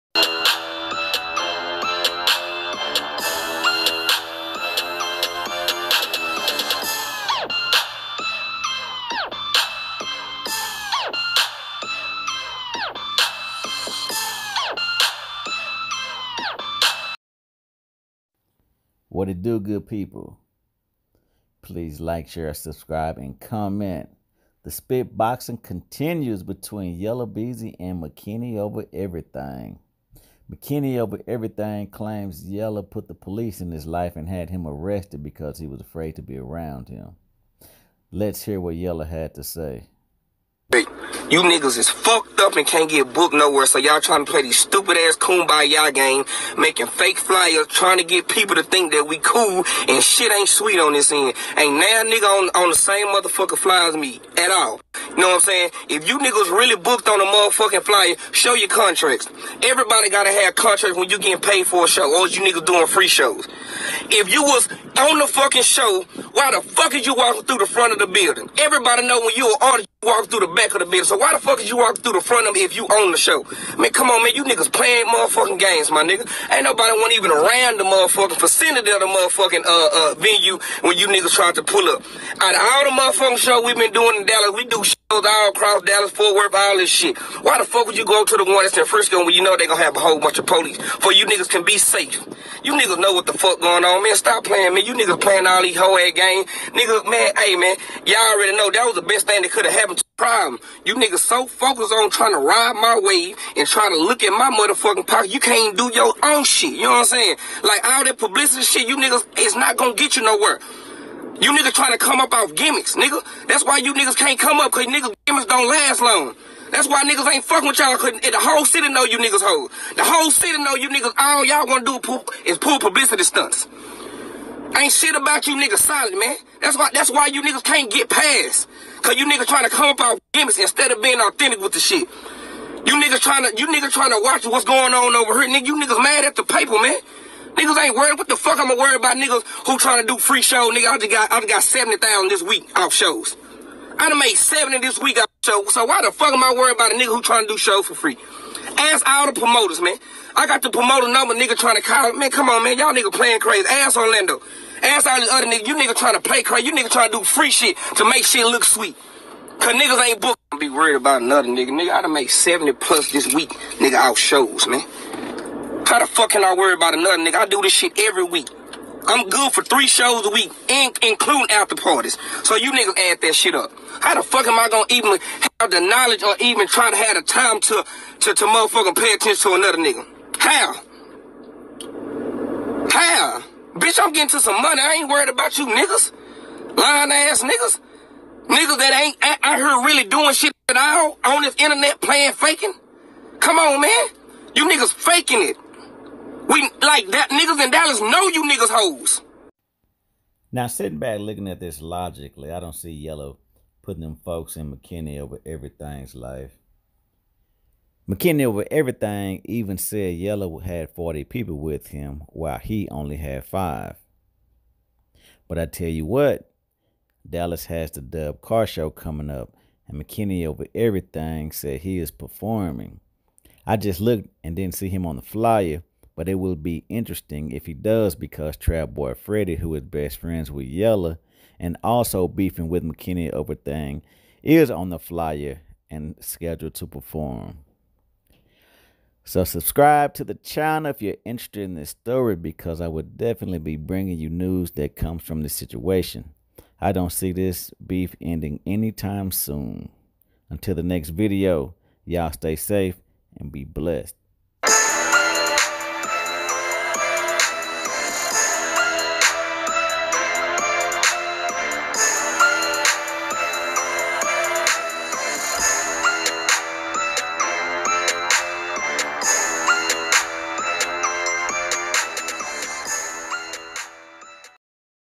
🔵YELLA EXPOSING MO3 FOR PERFORMING FOR FREE, PUBLICITY STUNTS AND RIDING HIS WAVE TRYING TO GET ON
According to YellaBezzy, Mo3 showed up to perform for free so he could get publicity off of Yella. McKinney Over Everything had to go in through the front when all rappers go in through the back. Yella said he wasn't even around when 3 got arrested at the venue.